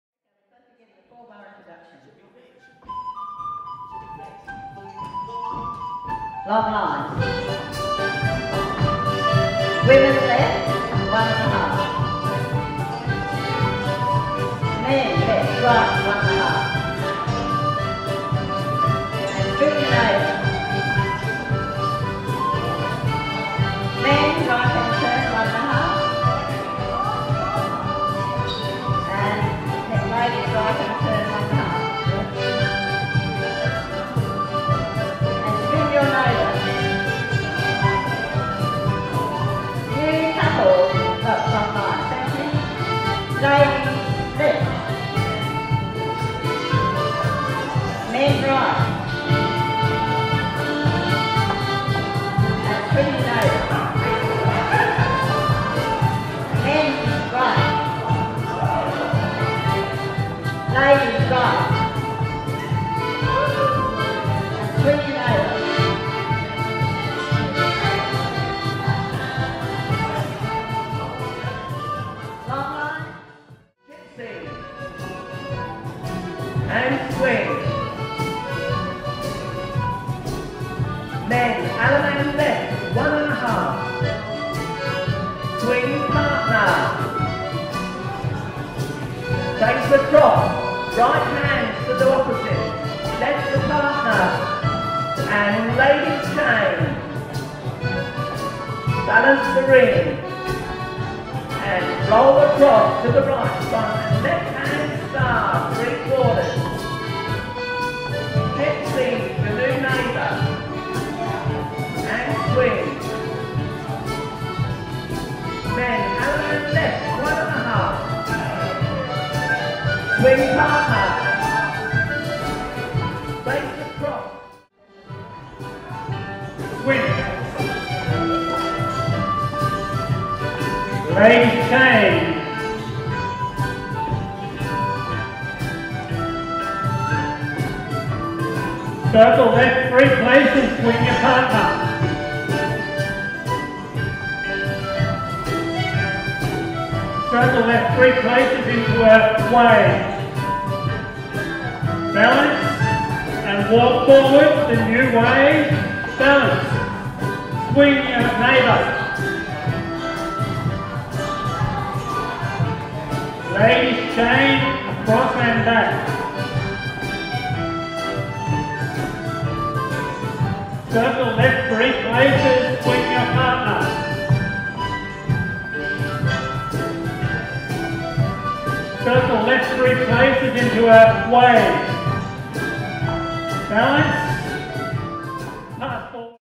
Contra Dance in Canterbury 2024
A Contra Dance was held on Saturday 27th January 2024 in Melbourne Australia.
 Dances were chosen with an Australian theme.
Music is by The Contradition Dance Band
Dances featured here are:
 Dust Devils by Keith Wood 2013
10:45 to Bungenore by Peter Foster 2007
Summer Sunshine by Paul Balliet 1992